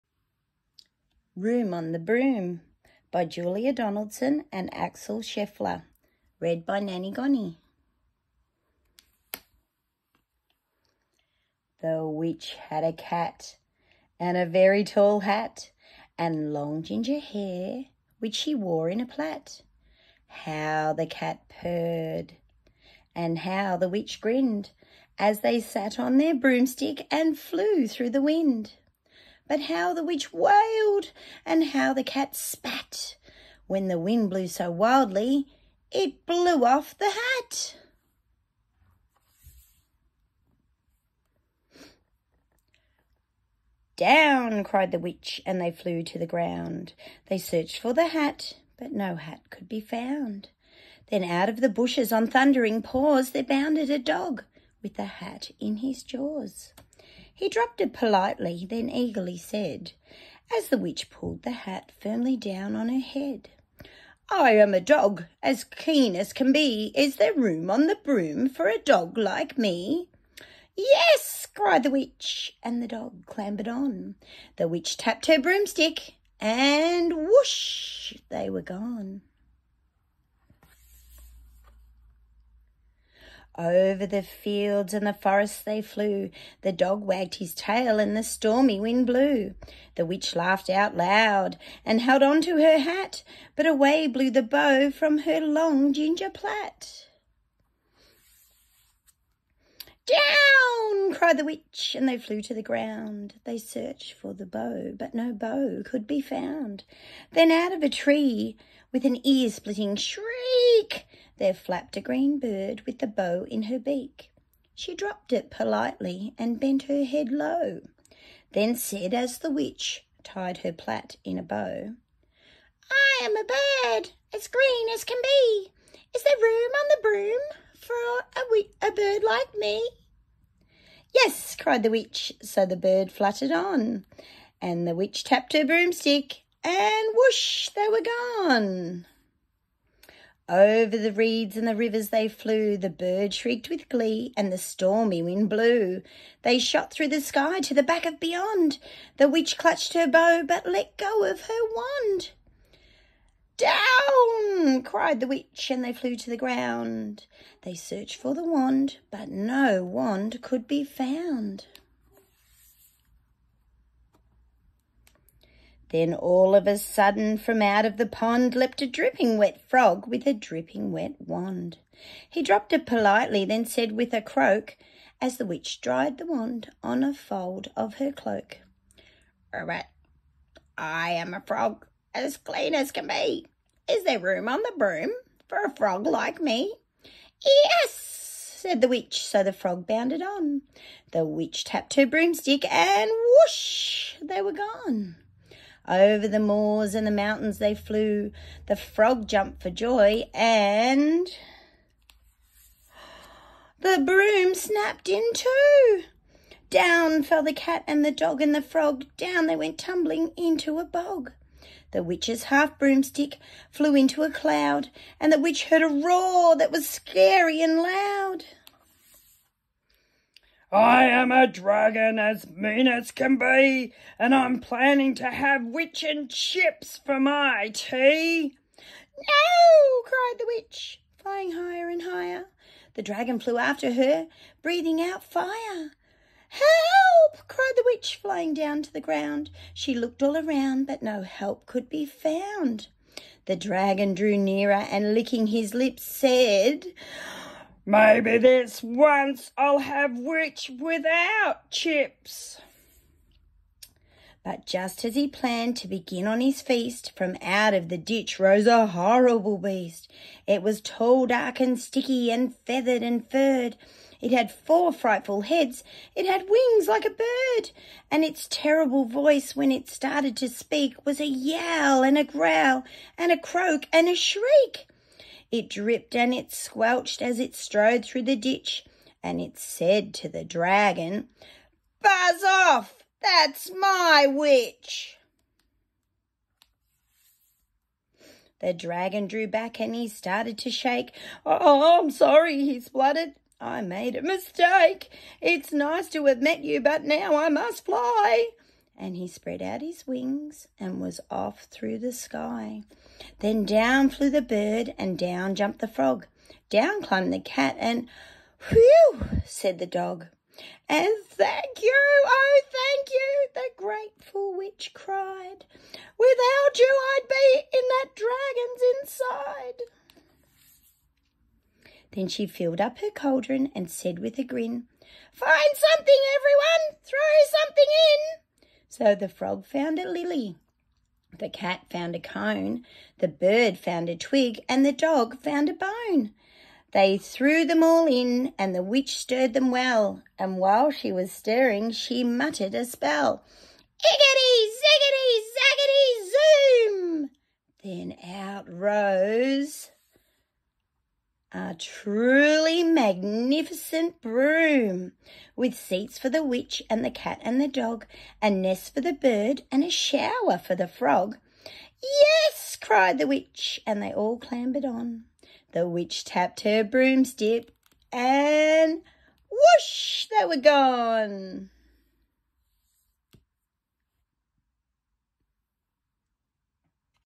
Room on the broom by Julia Donaldson and Axel Scheffler. Read by Nanny Gonny
Children’s book